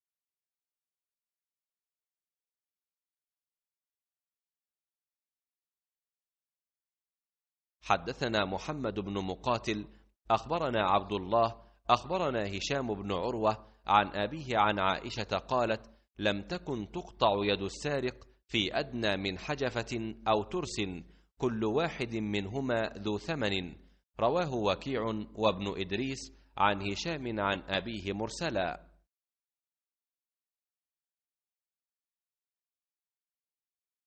صحيح البخارى - كتاب الحدود - 6793 - باب قول الله تعالى: {والسارق والسارقة فاقطعوا أيديهما}
حدثنا محمد بن مقاتل، أخبرنا عبد الله، أخبرنا هشام بن عروة، عن أبيه، عن عائشة، قالت لم تكن تقطع يد السارق في أدنى من حجفة أو ترس، كل واحد منهما ذو ثمن‏.‏ رواه وكيع وابن إدريس عن هشام عن أبيه مرسلا‏.‏